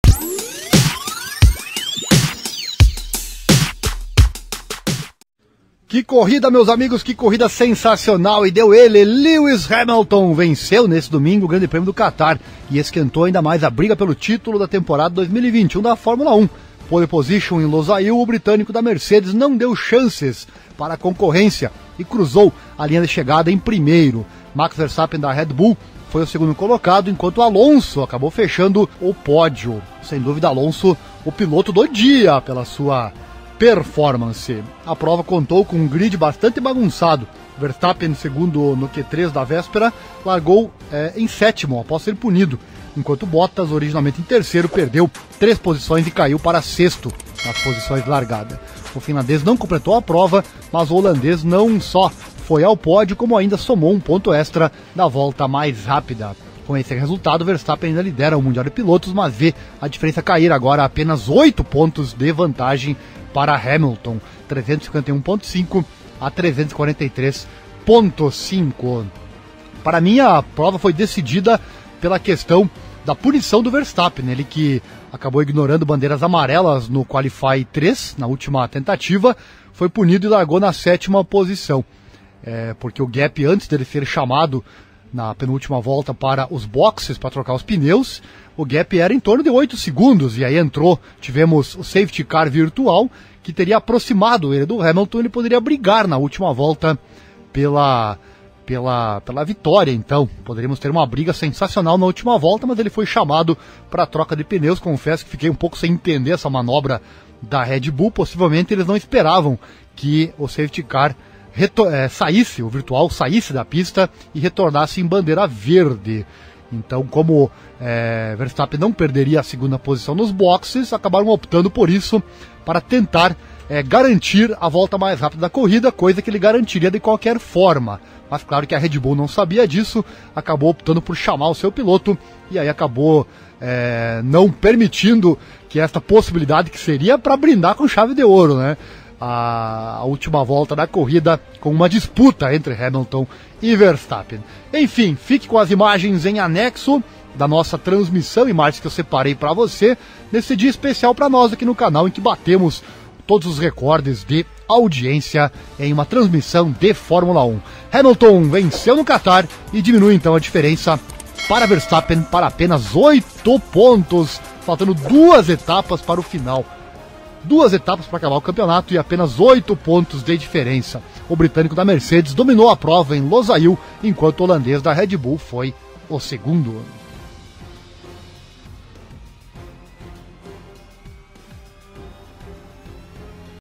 F1: Hamilton vence GP do Catar e diminui vantagem de Verstappen na briga pelo título
Britânico da Mercedes dominou a prova em Losail, enquanto holandês da Red Bull foi segundo